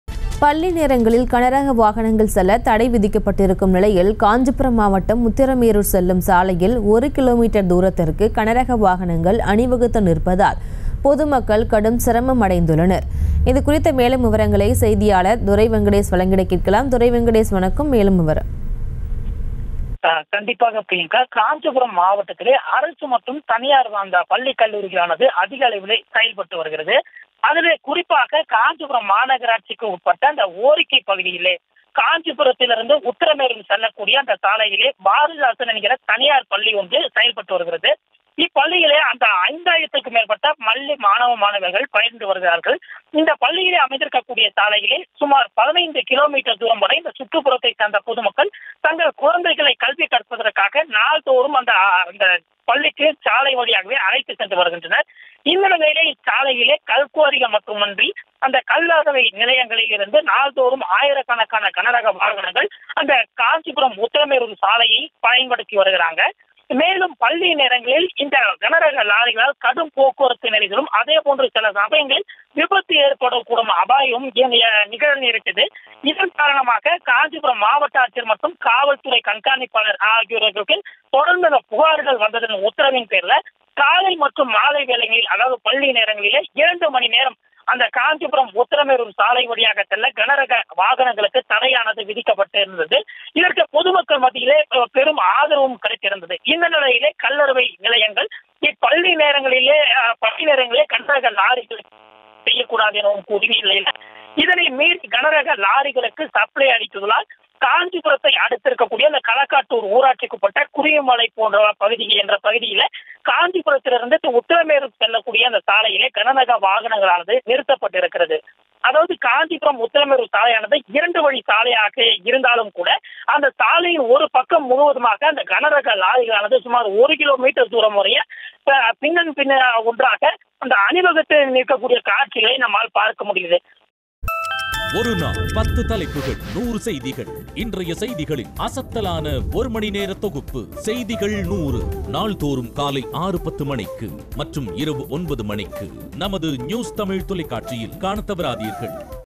அணிவகுத்து நின்ற கனரக வாகனங்கள்... ஸ்தம்பித்த போக்குவரத்து.. பொதுமக்கள் கடும் சிரமம் | Kanchipuram
அணிவகுத்து நின்ற கனரக வாகனங்கள்... ஸ்தம்பித்த போக்குவரத்து.. பொதுமக்கள் கடும் சிரமம் | Kanchipuram | Vehicles Lined Up | NewsTamil24x7